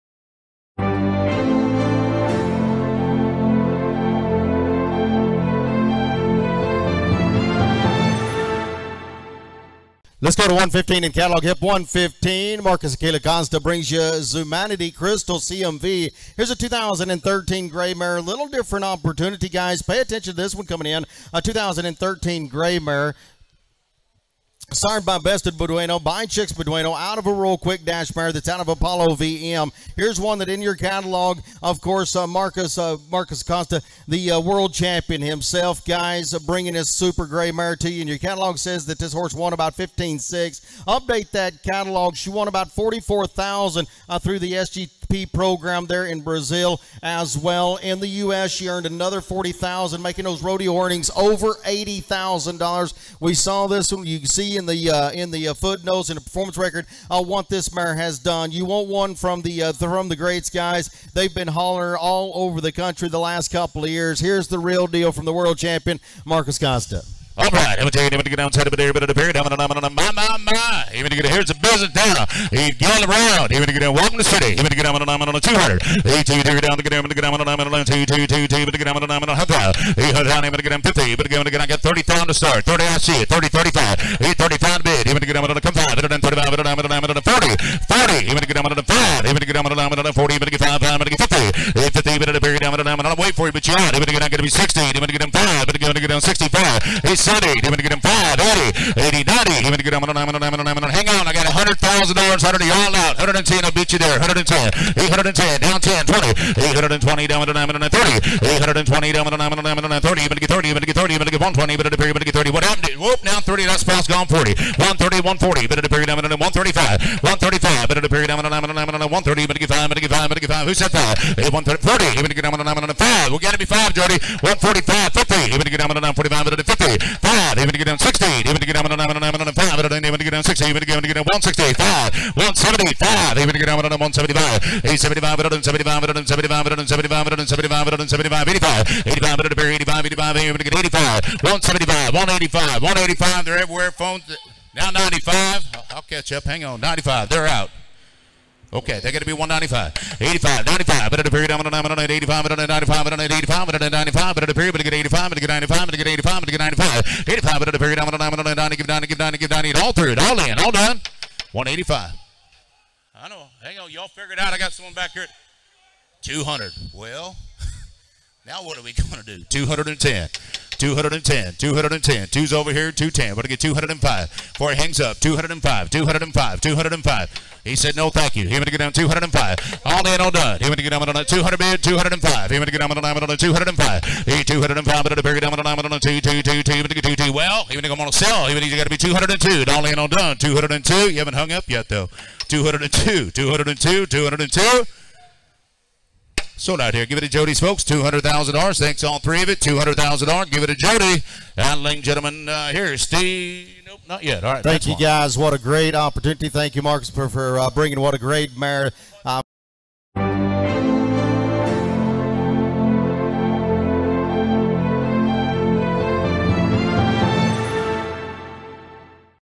Zumanity Cristal CMV Sells for $200K at Triangle Spring Sale 2023
Zumanity Cristal CMV 2013 Grey Mare sired by Best Of Beduino and out of Cristalquikdash01ibr Sells for $200,000 at the Triangle Spring Sale 2023